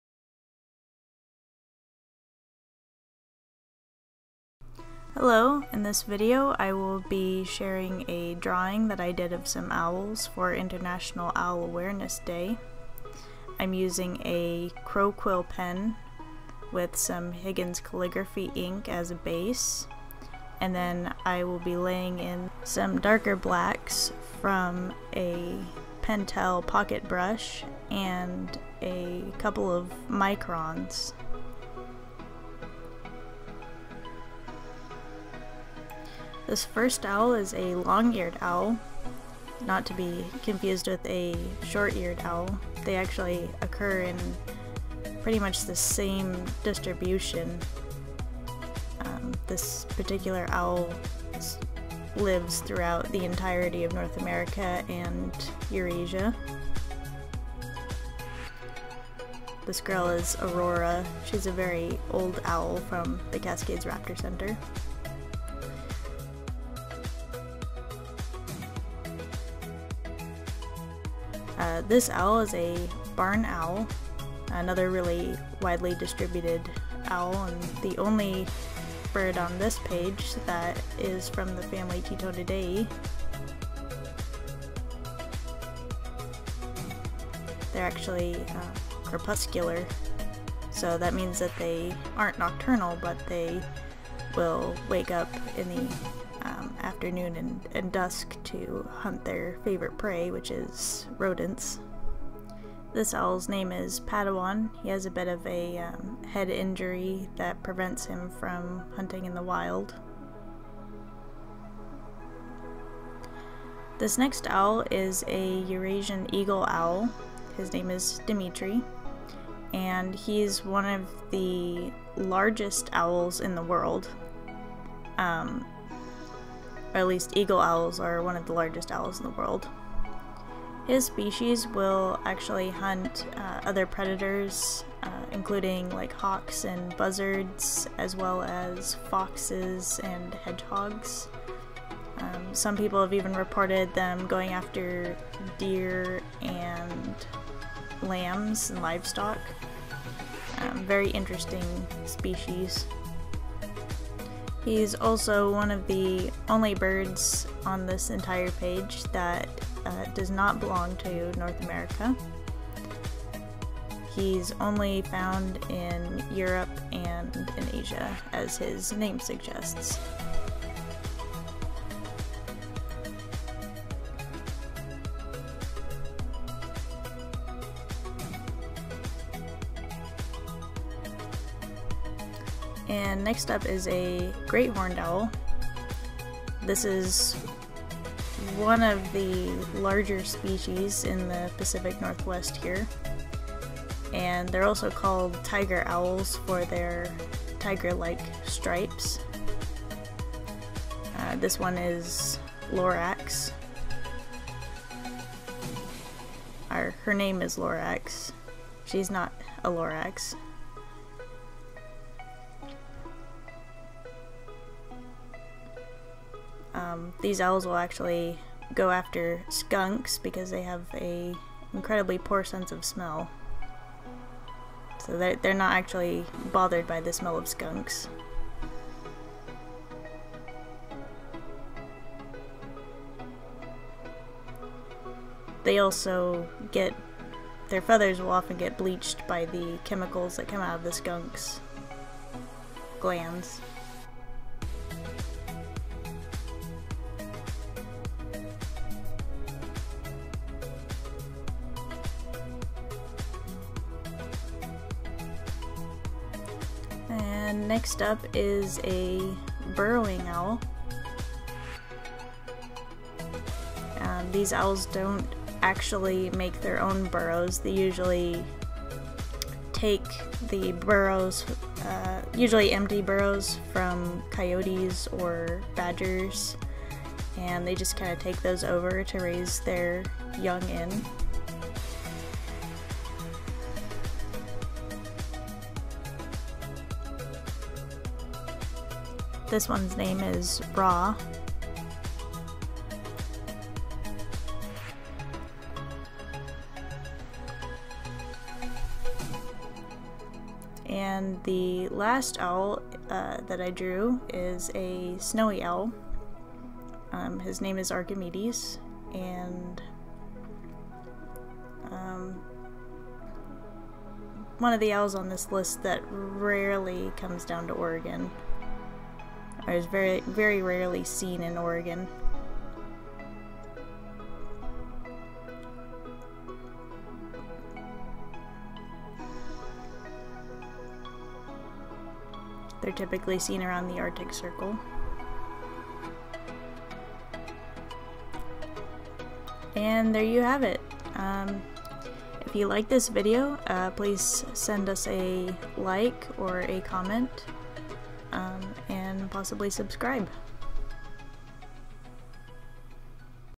Owl Drawing Time Lapse
Materials: Higgins Calligraphy ink, Pentel Pocket brush and Micron in Pentalic Travel Journal

Drawing based on personal photographs taken at the Cascades Raptor Center

Video has been edited for length.